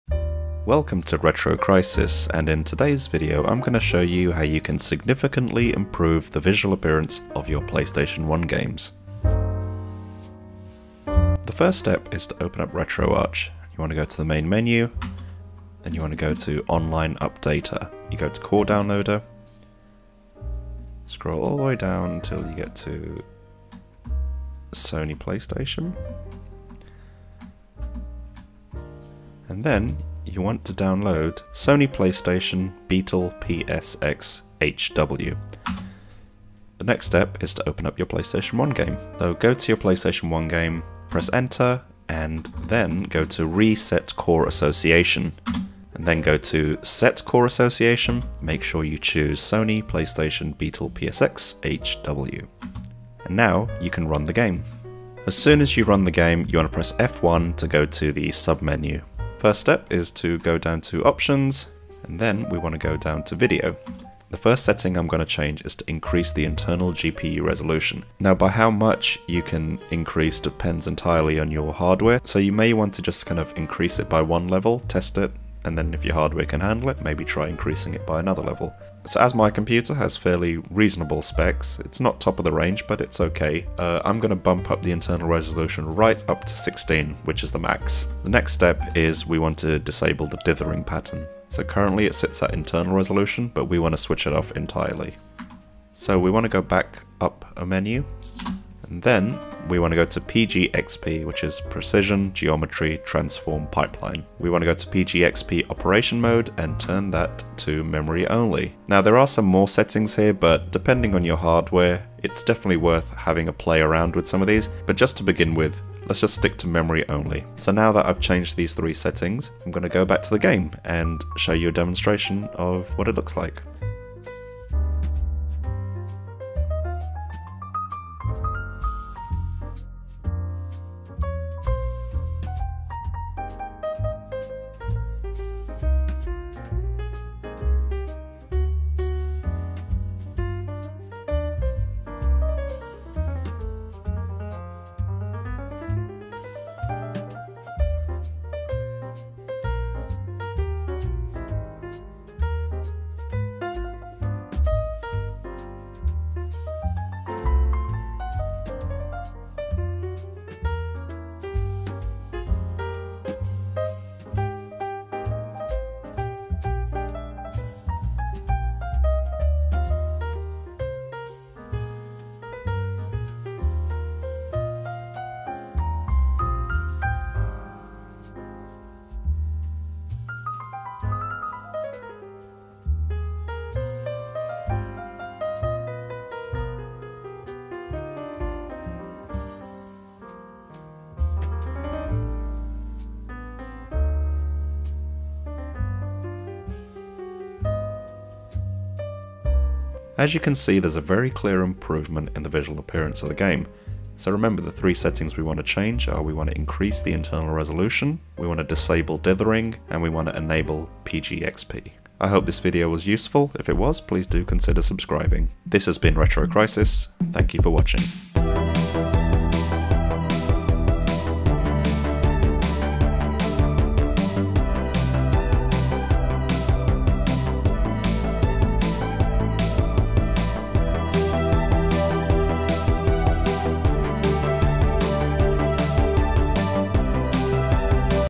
Sony PlayStation PS1 - 4K Emulation Guide - RetroArch PGXP
This is for the PSOne / PS1 / PlayStation platform

Beetle PSX HW is a port/fork of Mednafen's PSX module to the libretro API. It can be compiled in C++98 mode, excluding the Vulkan renderer, which is written in C++11 for the time being. Beetle PSX HW currently runs on Linux, OSX and Windows.

Notable additions in this fork are:
- PBP and CHD file format support, developed by Zapeth;
- Software renderer internal resolution upscaling, implemented by simias;
- An OpenGL 3.3 renderer, developed by simias;
- A Vulkan renderer, developed by TinyTiger;
- PGXP perspective correct texturing and subpixel precision, developed by iCatButler;

The Beetle PSX HW core has been authored by Mednafen Team

0:00 - Introduction to PS1 4K emulation
0:11 - Download Beetle PSX HW core for RetroArch
0:39 - Load a PS1 game with Beetle PSX HW
0:54 - Configuring PS1 games to run in 4K
1:05 - Increase PS1 Resolution from 240p to 4K
1:28 - Disable PS1 dithering pattern
1:39 - Enable PGXP Geometry correction for PS1 games
2:03 - PS1 Graphics comparison, 240p vs 4K with PGXP